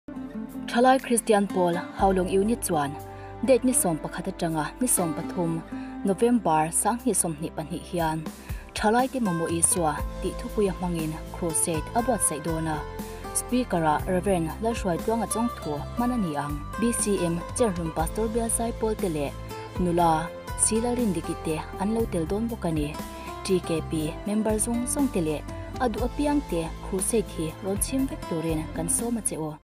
CRUSADE SAWMNA
Thalai Kristian Pawl Haulawng Unit chuan Dt 11-13 November 2022 hian THALAITE MAMAWH ISUA tih thupui a hmang in Crusade a buatsaih dawn a. Speaker ah Rev Lalhruaitluanga Chawngthu hman a ni ang. Dt 12 November 2022 Inrinni nileng in Biak in kawtah in tihhlimna hun hman bawk tur ani a. He hun min hmanpui tur hian BCM Cherhlun Pastor Bial Zaipawl te leh Nl C Lalrindiki  te an lo tel dawn bawk a ni.

TKP Member zawng zawng te leh a duh apiang te Crusade hi rawn chhim vek turin kan sawm a che u